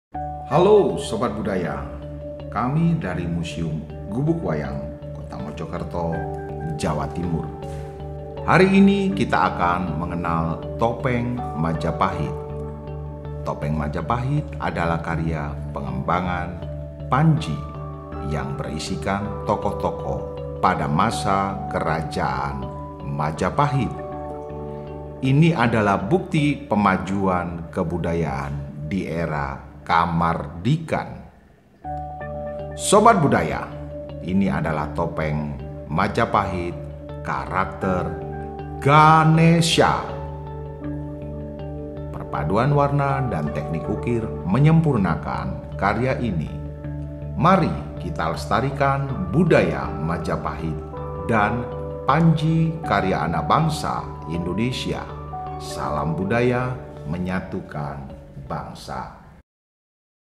TOPENG MAJAPAHIT EPS 19
Perkenalkan ini adalah Topeng Majapahit. Ini adalah pengembangan topeng panji yang berisikan tokoh-tokoh Majapahit. Topeng ini saat ini ada dilokasi Museum Gelar Indonesia Budaya (Ganesya) yang berlokasi di Malang tepatnya di area Hawai Waterpark, Malang. Mau tau lebih lanjut bagaimana bisa datang ke Malang ya! 😊